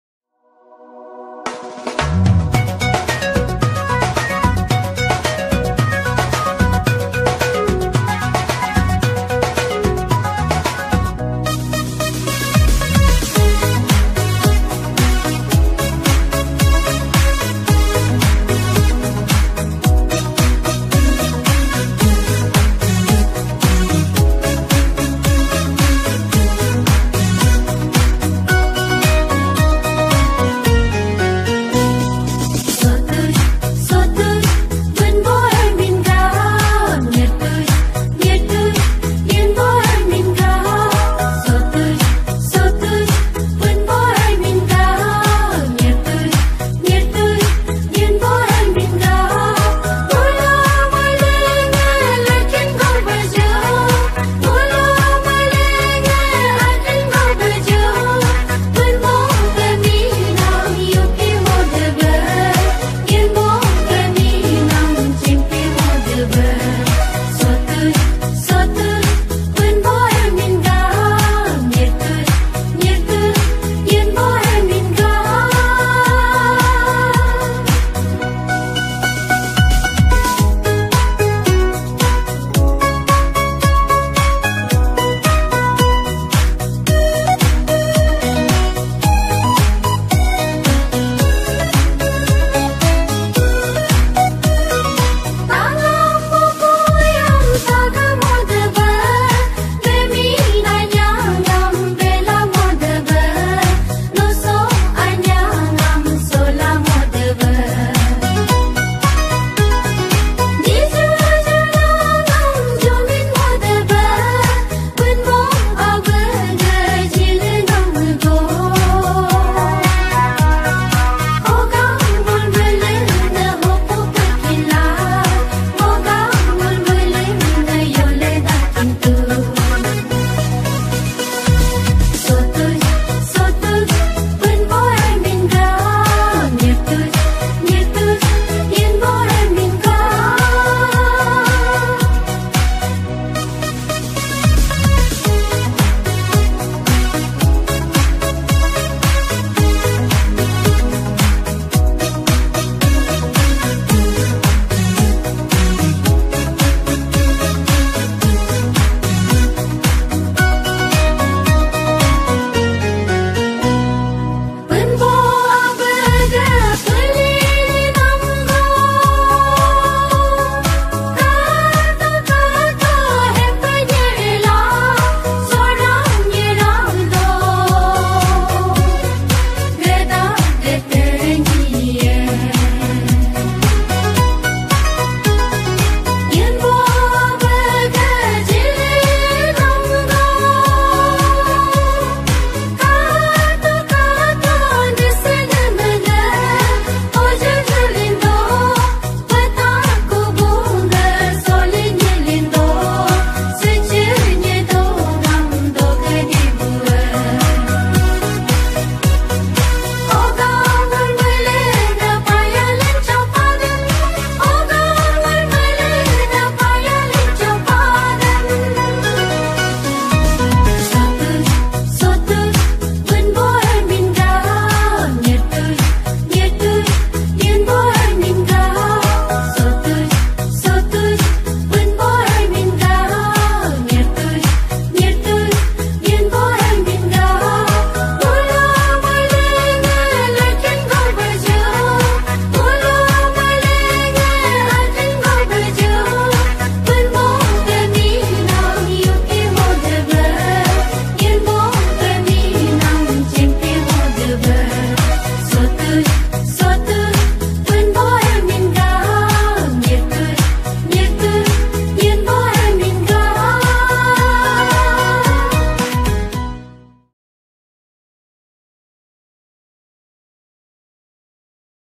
Nyishi Christian Dance/ Nyishi Baptist Church Council
Beautiful Dance during 4th Balijan Pastoral Range Annual Fellowship by women Balijan Town Baptist